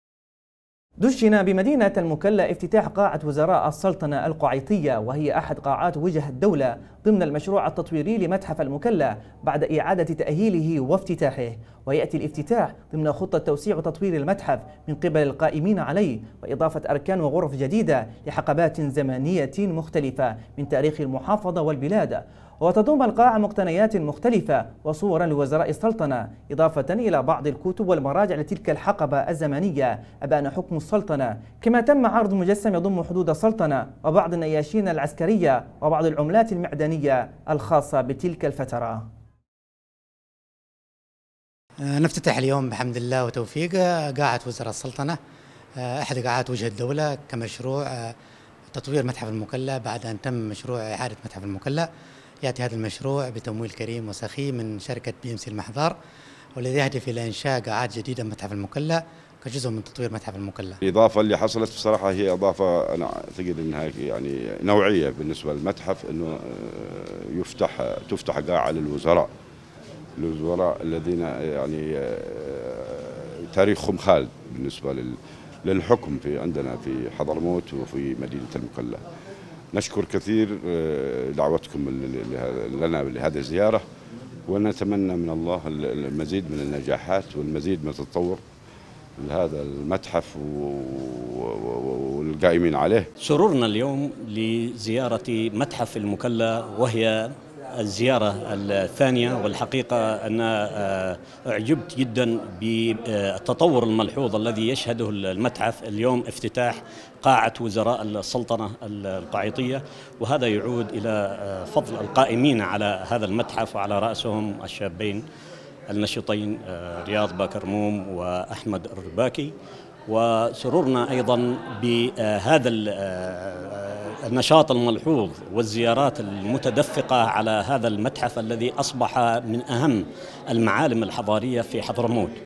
خبر | افتتاح قاعة وزراء السلطنة القعيطية بمتحف المكلا
دُشن بمدينة المكلا افتتاح قاعة وزراء السلطنة القعيطية وهي أحد قاعات وجه الدولة ضمن المشروع التطويري لمتحف المكلا بعد إعادةِ تأهيله وافتتاحه